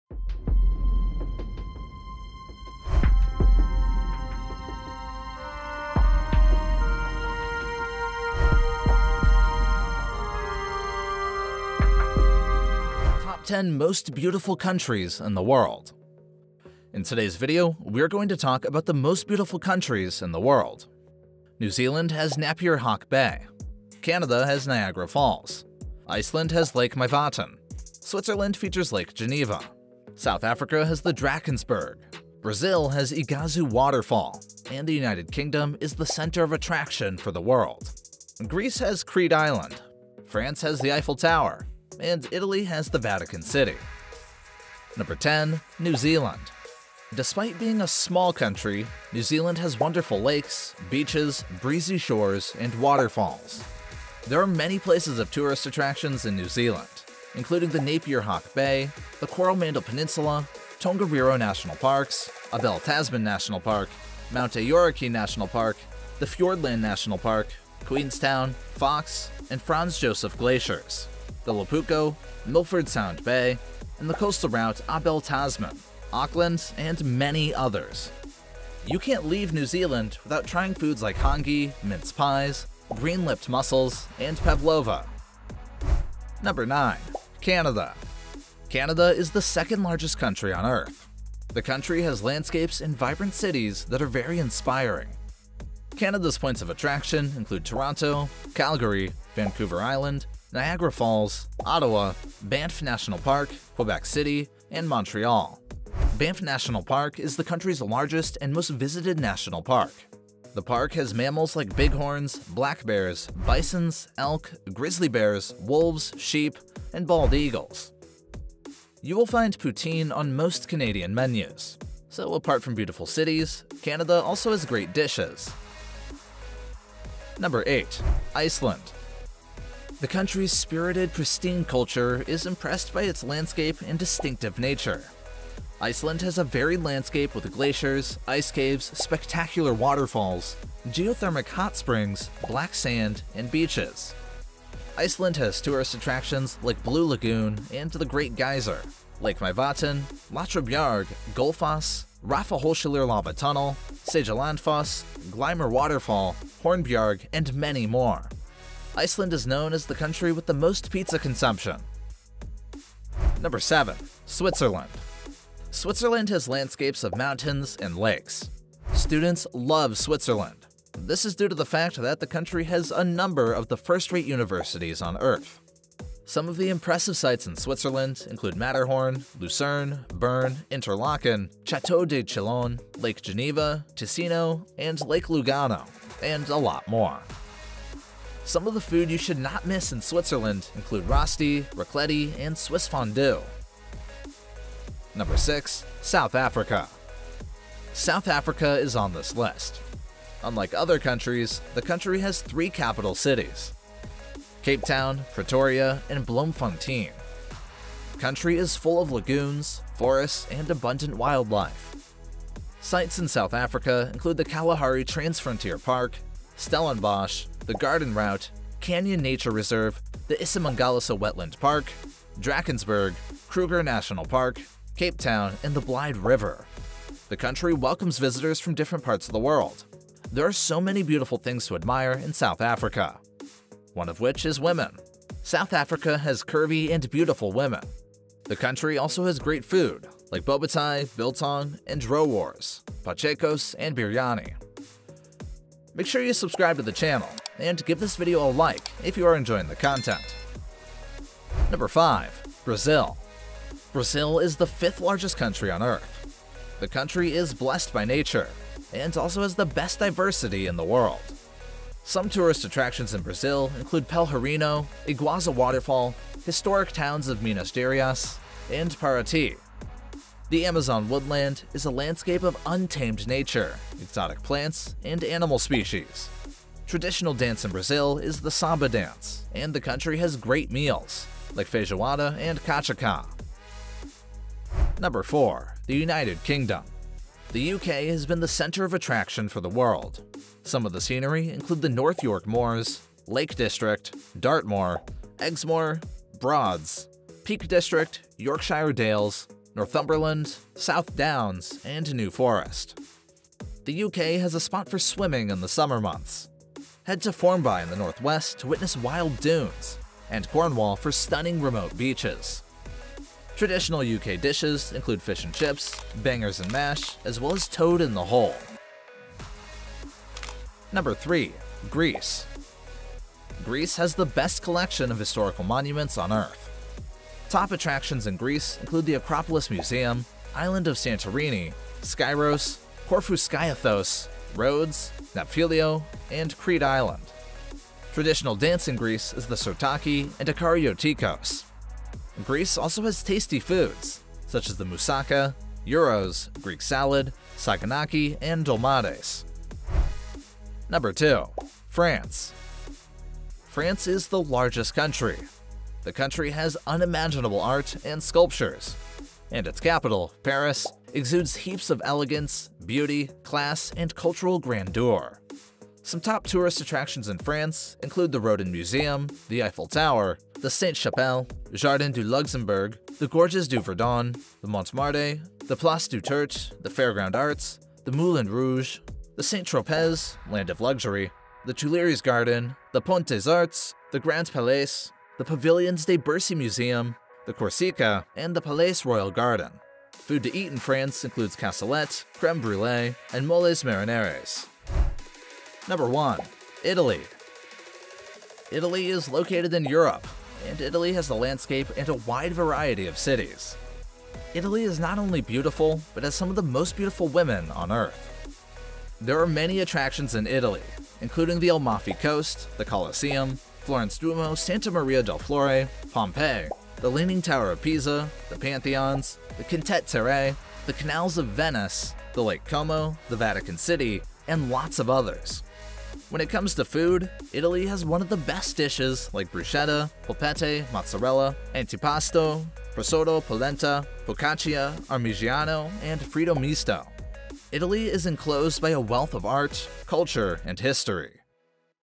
Top 10 Most Beautiful Countries in Europe
In today’s video, we are going to talk about the most beautiful countries in the world. New Zealand has Napier Hawke Bay, Canada has Niagara Falls, Iceland has Lake Myvatn, Switzerland features Lake Geneva, South Africa has the Drakensberg, Brazil has Iguazu waterfall, and the United Kingdom is the center of attraction for the world., Greece has Crete Island. France has The Eiffel Tower, and Italy has the Vatican city.

10. New Zealand
Despite being a small country, New Zealand has wonderful lakes, beaches, breezy shores, and waterfalls. There are many places of tourist attractions in New Zealand including the Napier Hawke Bay, the Coromandel Peninsula, Tongariro National Parks, Abel Tasman National Park Mount Aoraki National Park,

9. Canada
Canada is the second largest country on earth. The country has landscapes and vibrant cities that are very inspiring. Canada’s points of attraction include Toronto, Calgary, Vancouver Island, Niagara Falls, Ottawa, Banff National Park, Quebec City, and Montreal.

8. Iceland
The country’s spirited pristine culture is impressed by its landscape and distinctive nature. Iceland has a varied landscape with glaciers, ice caves, spectacular waterfalls, geothermic hot springs, black sand, and beaches.

7. Switzerland
 Switzerland has landscapes of mountains and lakes. Students love Switzerland. This is due to the fact the country has a number of the first-rate universities on earth, Some of the impressive sights in Switzerland include The Matterhorn, Lucerne, Bern,

6. South Africa
South Africa is on this list. Unlike other countries, The country has three capital cities: Cape Town, Pretoria, and Bloemfontein.The country is full of, lagoons, forests, and abundant wildlife. Sights in South Africa include the Kalahari Transfrontier Park, Stellenbosch, the Garden Route, Canyon nature reserve, the Isimangaliso Wetland Park, Drakensberg, Kruger National Park, Cape Town, and the Blyde River, The country welcomes visitors from different parts of the world.

5. Brazil
Brazil is the fifth largest country on earth. The country is blessed by nature, and also has the best diversity in the world. Some tourist attractions in Brazil include Pelhourinho, Iguazu waterfall Historic towns of Minas Gerais and Parati. The Amazon woodland is a landscape of untamed nature, exotic plants, and animal species.

4. The United Kingdom
The UK has been a center of attraction for the world. Some of the scenery include the: North York Moors, Lake District, Dartmoor, Exmoor, Broads, Peak District, Yorkshire Dales, Northumberland, South Downs, and New Forest.

3. Greece
Greece has the best collection of historical monuments on earth. Top attractions in Greece include the Acropolis Museum, Island of Santorini, Skyros, Corfu Skiathos, Rhodes, Nafplio, and Crete Island.

2. France
 France is the largest country, the country has unimaginable art and sculptures, and its capital, Paris, exudes heaps of elegance, beauty, class, and cultural grandeur.

1. Italy
Italy is located in Europe, and Italy has a Landscape and a wide variety of cities, Italy is not only beautiful, but it also has some of the most beautiful women on the earth.